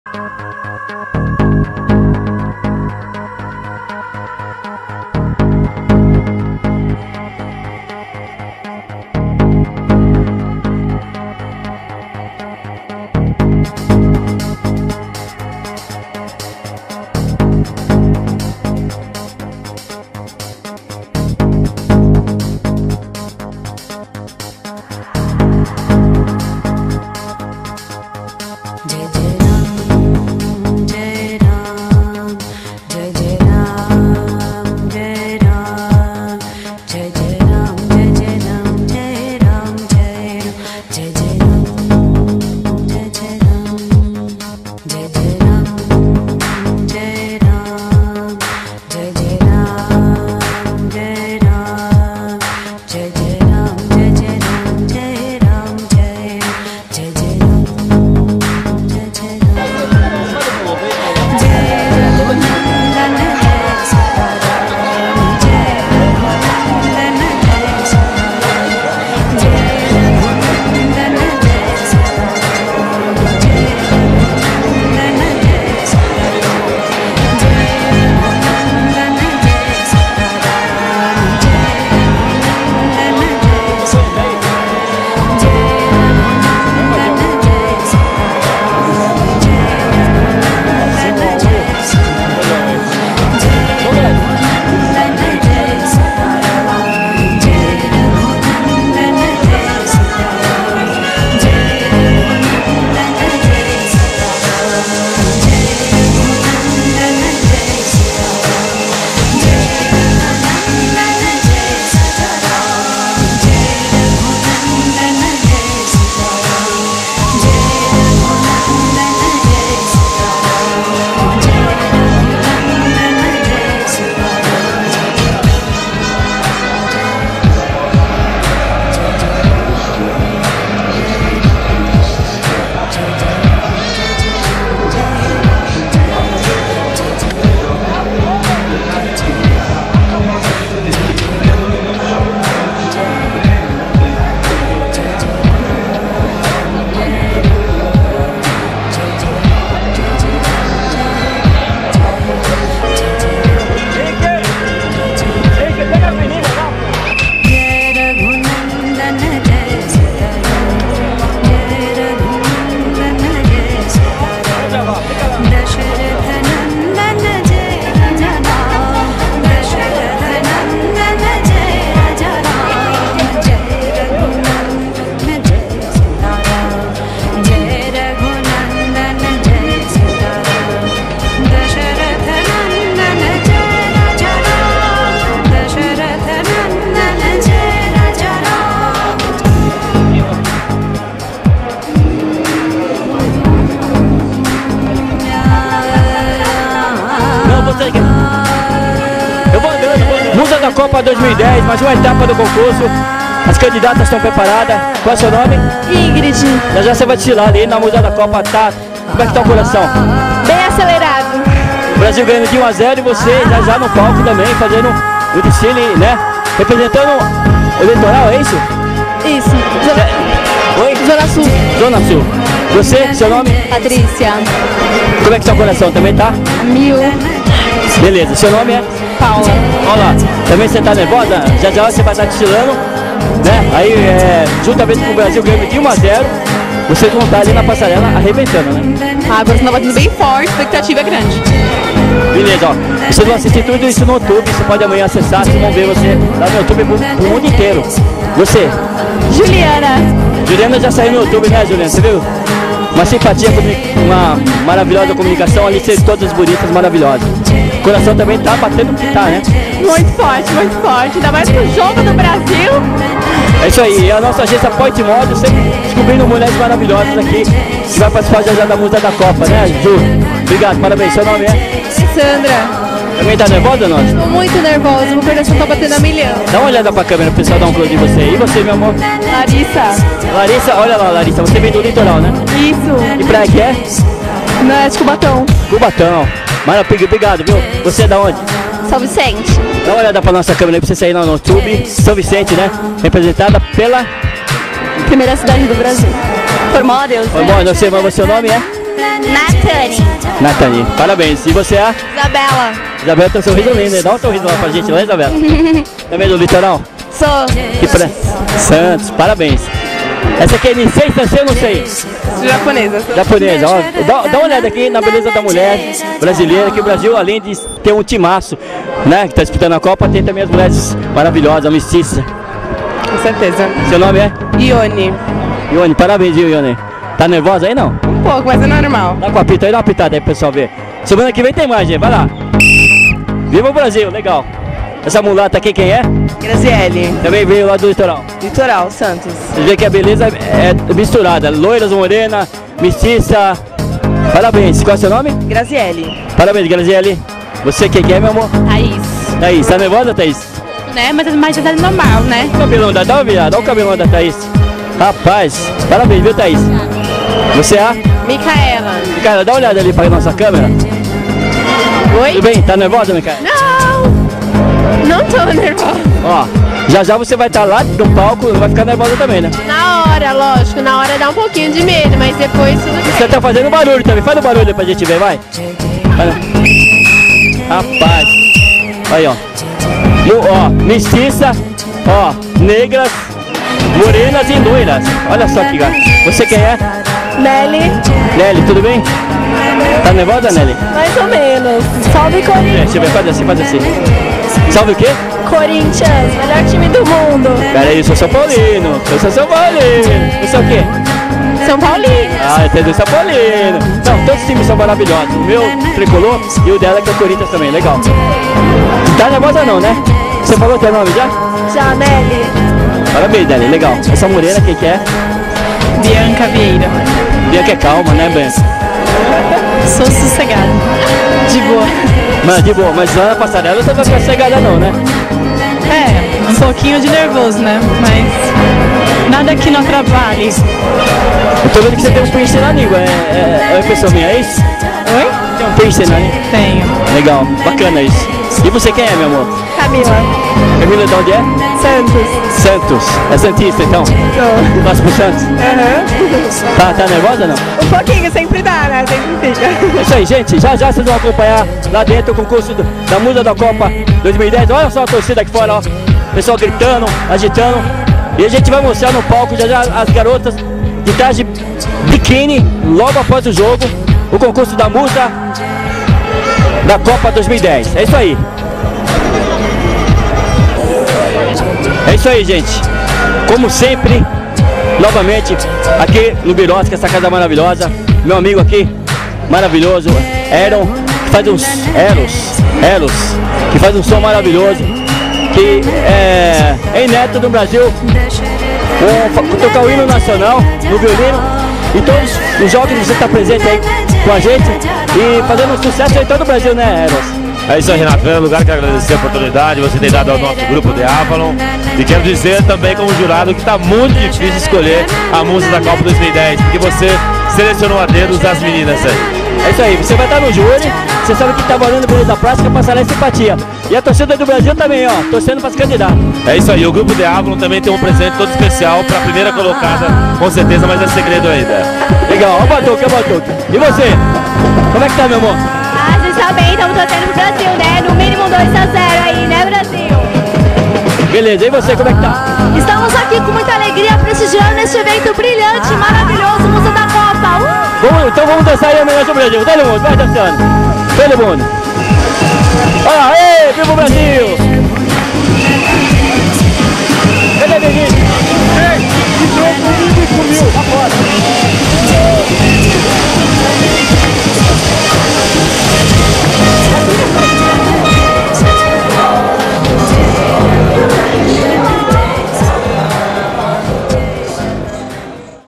MUSA DA COPA - 2ª ETAPA - APRESENTAÇÃO DAS CANDIDATAS.mp4
APRESENTAÇÃO DAS CANDIDATAS AO TITULO DE MUSA DA COPA, NO BASTIDORES (SP) NO DIA DO JOGO ENTRE BRASIL 3X1 COSTA DO MARFIM